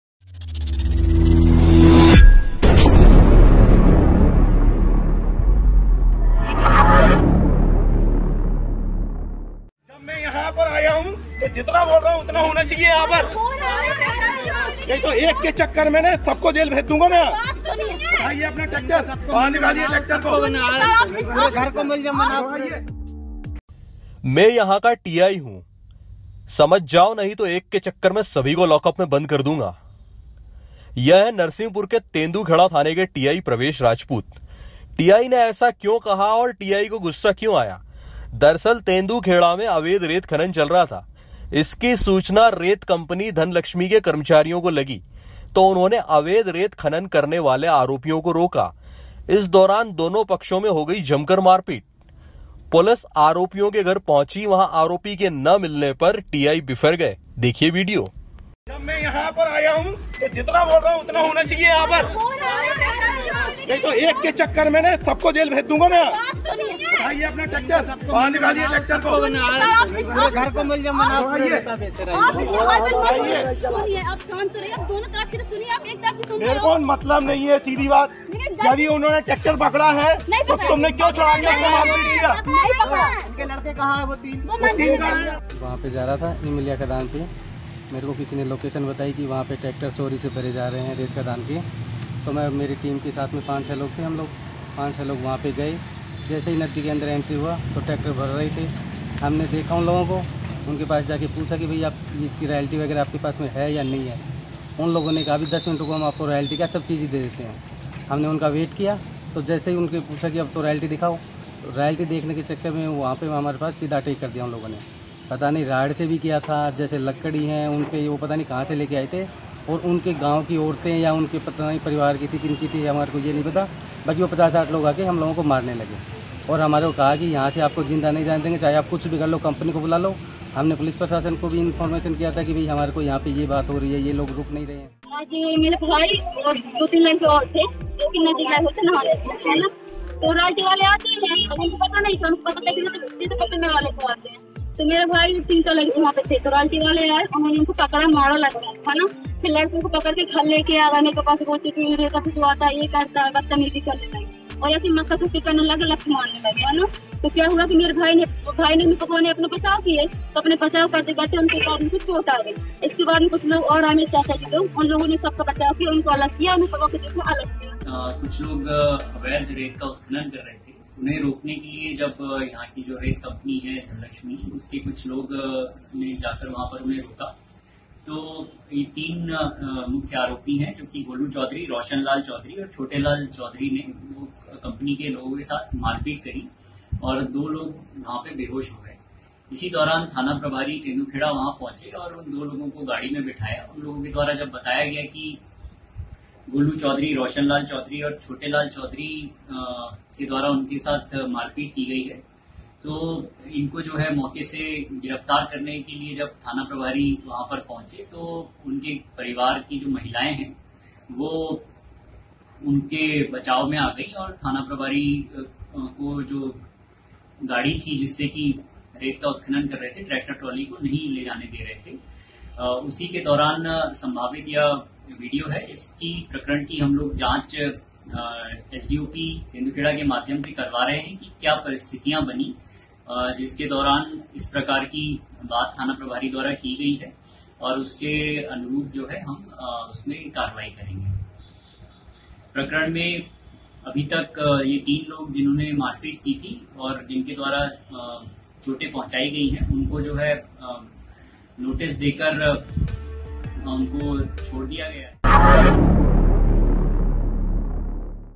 मैं यहाँ का टीआई हूँ ठीक कर दूंगा | The Xpose Today news
वीओ
मैं यहाँ का टीआई हूं लड़का नहीं मिला तो सबको लॉक अप में  बंद कर दूंगा यह हैं नरसिंहपुर के तेंदूखेडा थाने के टीआई प्रवेश राजपूत । दरअसल यहाँ अवैध रेत खनन चल रहा था इसकी सूचना रेत कंपनी धन लक्ष्मी के कर्मचारियों को लगी तो उन्होंने आरोपियों को रोका और इस बात पर दोनों पक्षों में हो गई जमकर मारपीट।पुलिस आरोपियों के घर पहुंची वहाँ आरोपी के न मिलने पर टीआई बिफर गए ।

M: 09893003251